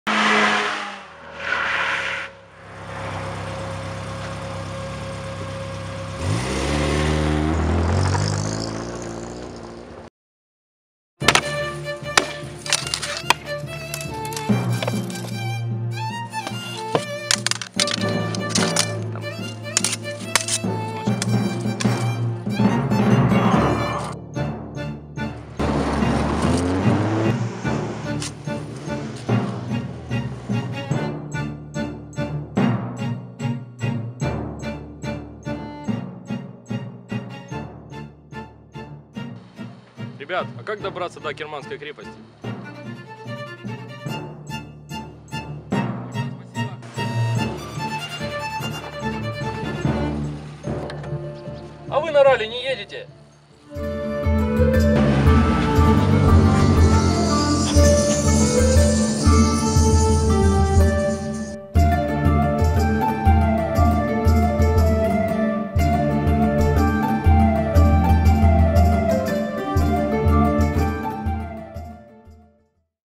Ралли Аккерман. Приглашение на этап Кубка Лиманов от команды Василия Кобенка
17-18 октября 2015 года в Белгород-Днестровском состоится 4-й этап Чемпионата Украины по мини-ралли Кубок Лиманов. Автогонщики Василий Кобенок и Олег Пантелей примут участие в этапе и будут бороться за чемпионство в классе Р-6.
17 октября с 17:00 состоится открытие ралли в Аккерманской крепости.

Информационный партнеры сайт "СПОРТ-Фото". И паблик в ВК Одесса улыбка Б-га